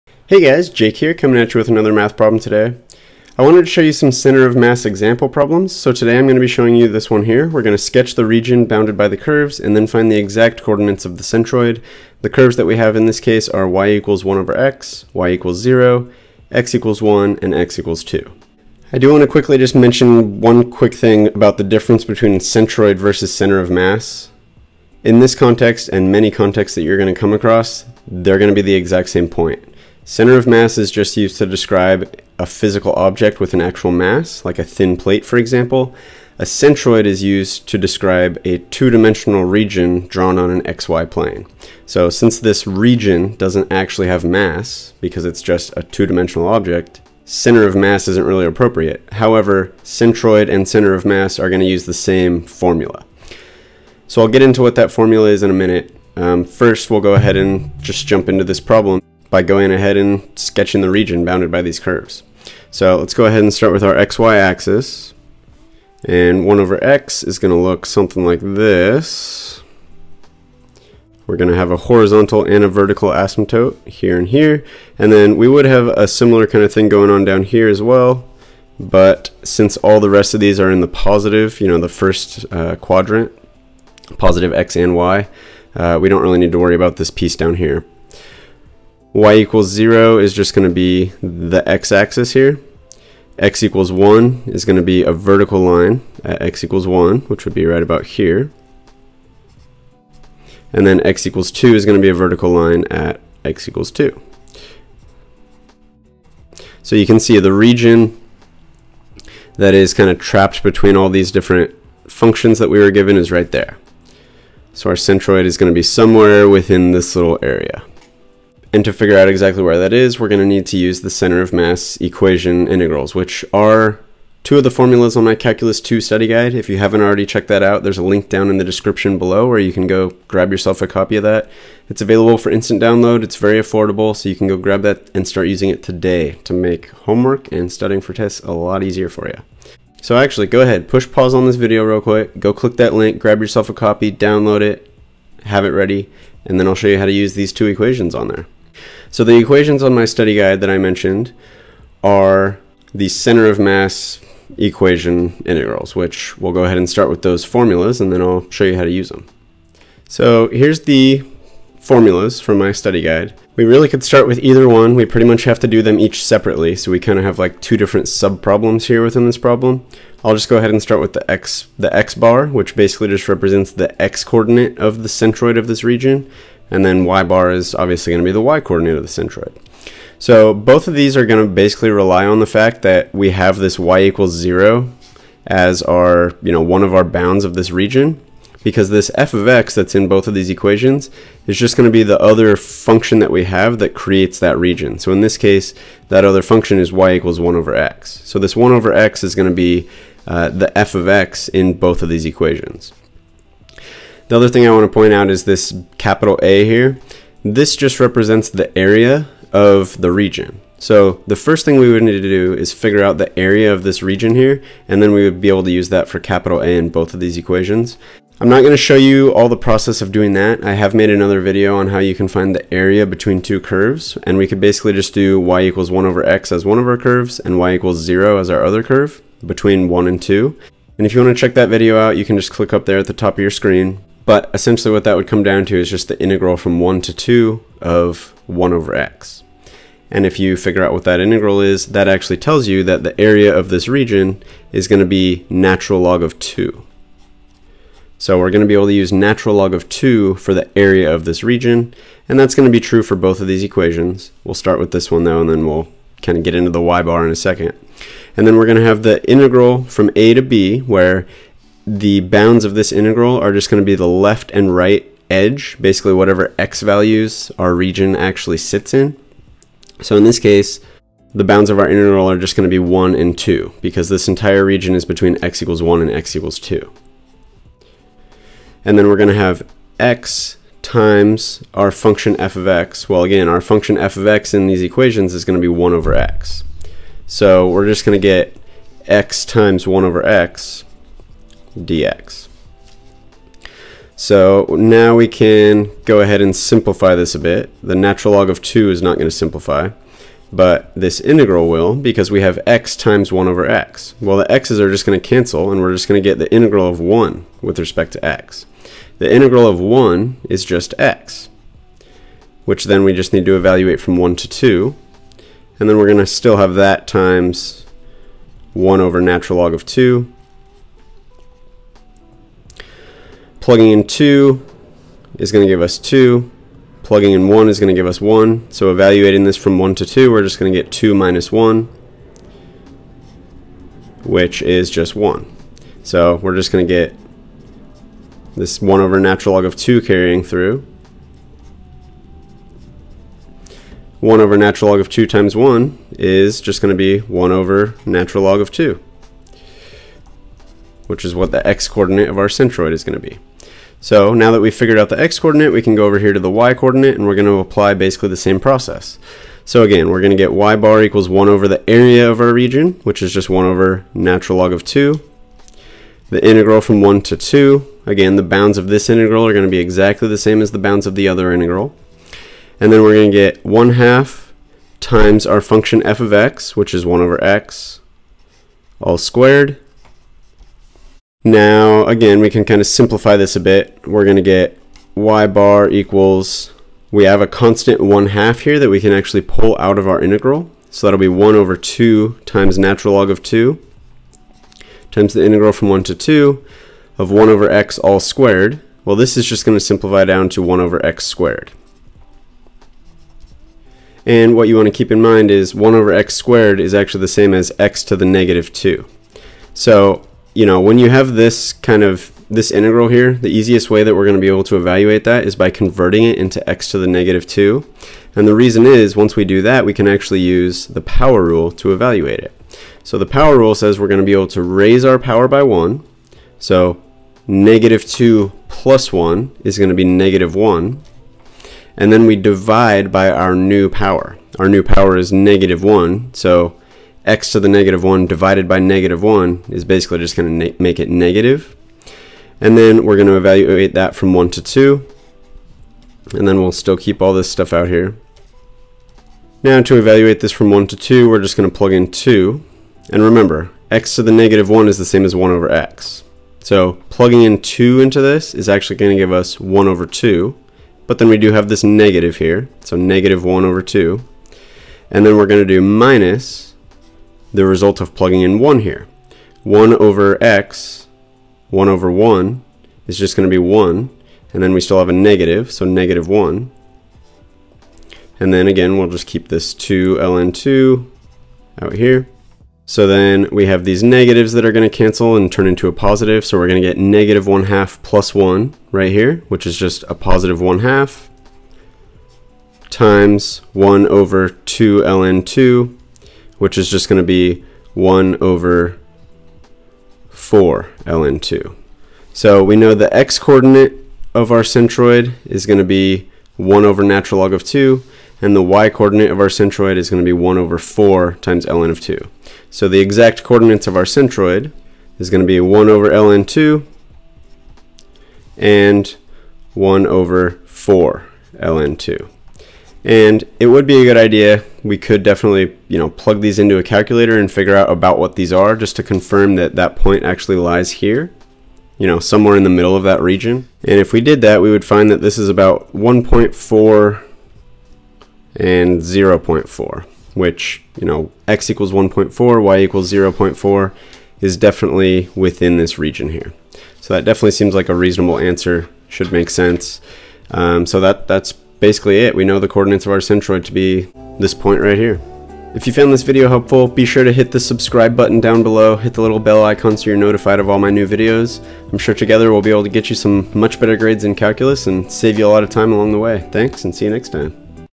CENTROID OF THE REGION - HOW TO FIND, y=1/x, y=0, x=1, x=2 | Center of mass equation integral
This video kicks off my center of mass example problems.  Sketch the region bounded by the curves, then find the exact coordinates of the centroid.  The given curves are: y=1/x, y=0, x=1, and x=2.  I will show you how to find the centroid of this region using the center of mass equation integral from my calculus 2 study guide, while also discussing the difference between centroid vs center of mass.

0:00 Center of mass example problems
0:18 Centroid vs Center of mass
0:56 Sketch the region bounded by the curves
1:58 Center of mass equation integral
4:00 Apply the center of mass formula

+ Problem Credit: Single Variable Calculus: Concepts and Contexts by James Stewart - Chapter 6.6 Problem #48